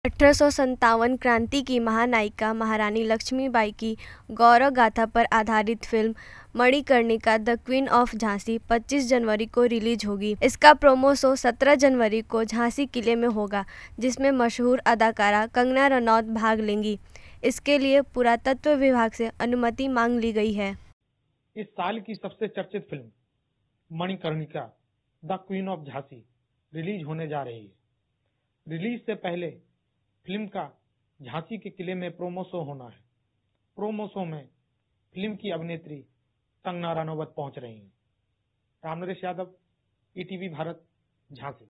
17 जनवरी को 'मणिकर्णिका' के प्रोमो-शो के लिए झांसी आएंगी कंगना रनौत
झांसी : बॉलीवुड की मशहूर अदाकारा कंगना रनौत की अपकमिंग मूवी 'मणिकर्णिका: द क्वीन ऑफ झांसी' इन दिनों चर्चा में है. फिल्म 25 जनवरी को रिलीज होने वाली है. फिल्म रिलीजिंग से पहले कंगना 17 जनवरी को झांसी पहुंचेंगी और यहां प्रोमो शो में भाग लेंगी.